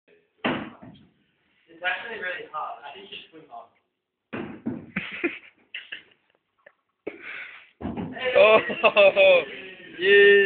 Numchuck cricket.
David trying to hit a rope ball with numchucks.

3rd times a charm.